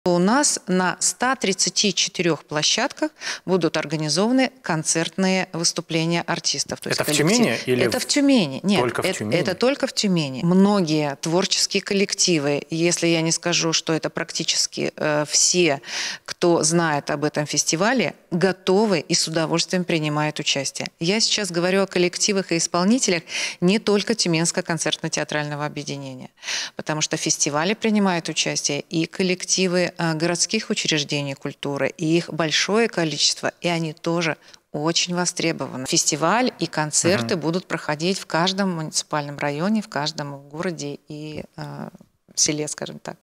Фестиваль на всю область
Фестиваль "Тюменская осень" объединит всех жителей региона. Артисты готовятся дарить гостям праздничное настроение. Сколько концертных площадок будет работать и кого зрители увидят на сцене?